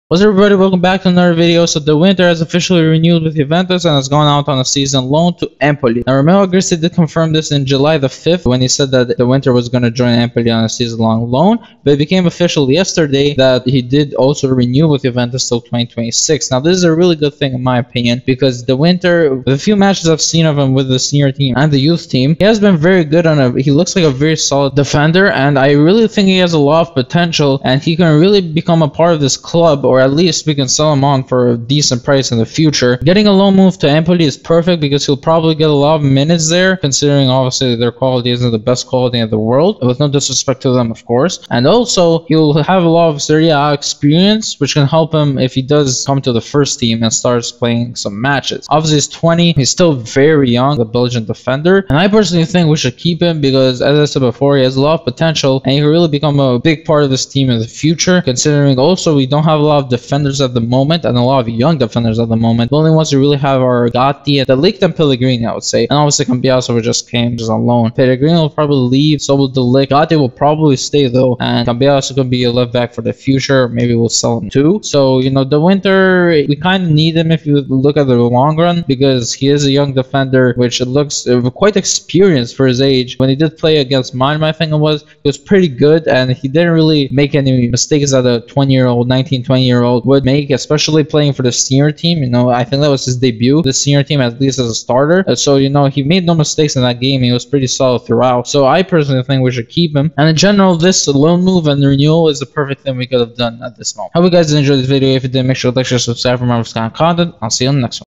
De Winter renews for Juventus and goes out on loan to Empoli!
Yo guys so today I talked about how De Winter renewed for Juventus and has now gone out on loan to Empoli. I hope y'all enjoy this video!